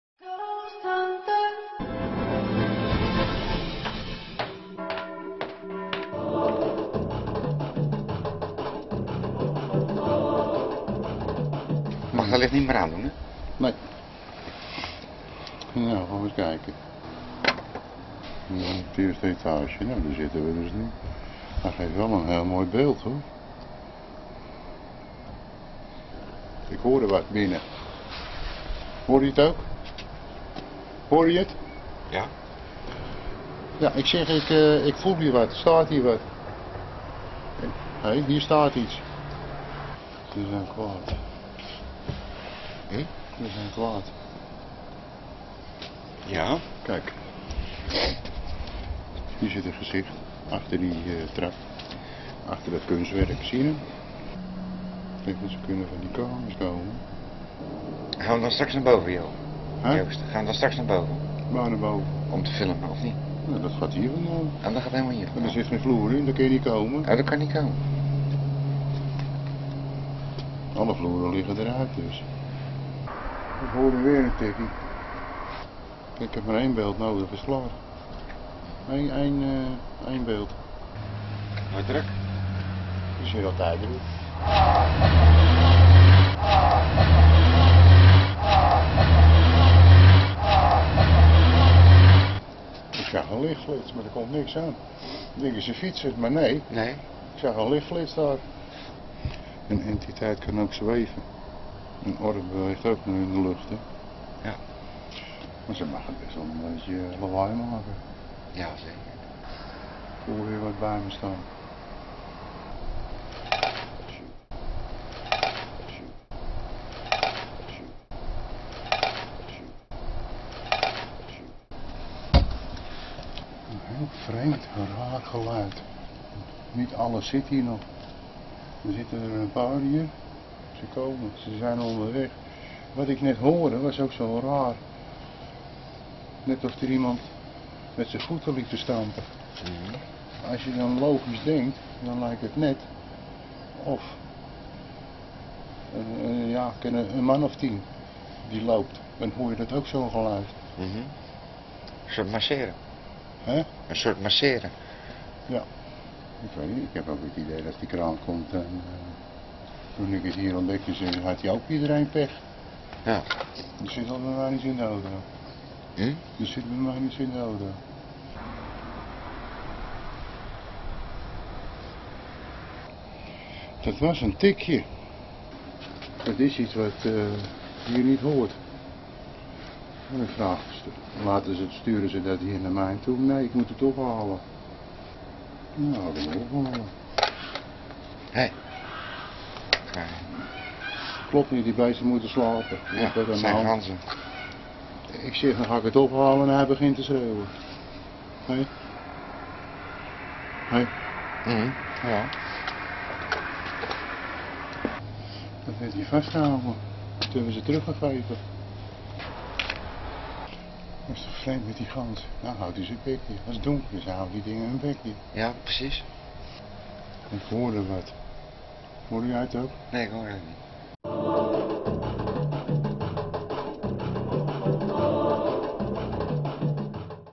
Laatste Nachtfilm Sas van Gents Spookhuis
Joost-The Ghosthunter gaat zijn laatste nachtfilm maken in het Spookhuis van Sas van Gent. En zoals het een spookhuis betaamt komen er rare dingen en geluiden op de film.
De laatste nacht.